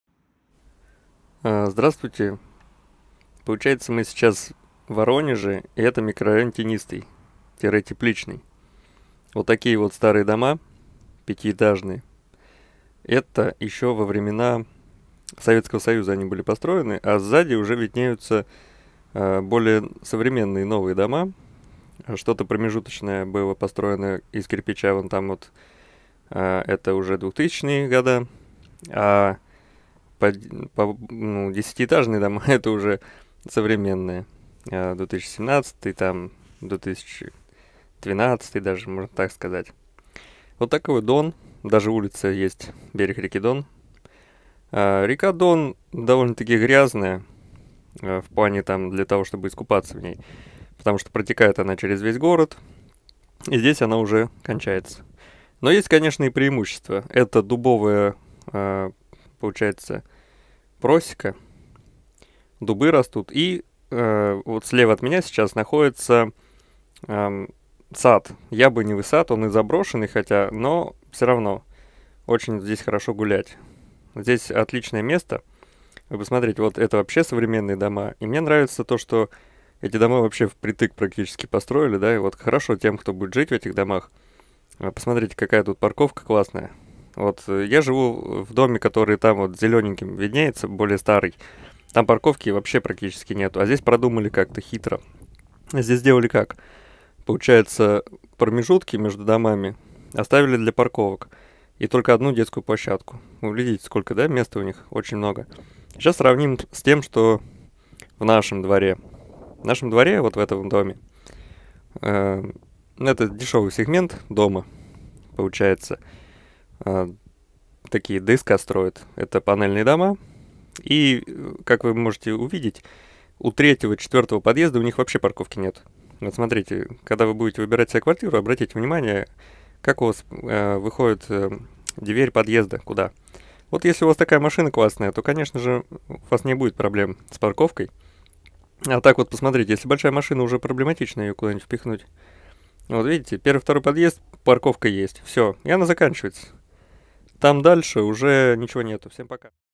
Тенистый микрорайон Воронежа!
В городе несколько районов и небольших микрорайонов. Планирую записать видео на городскую жизнь в Воронеже!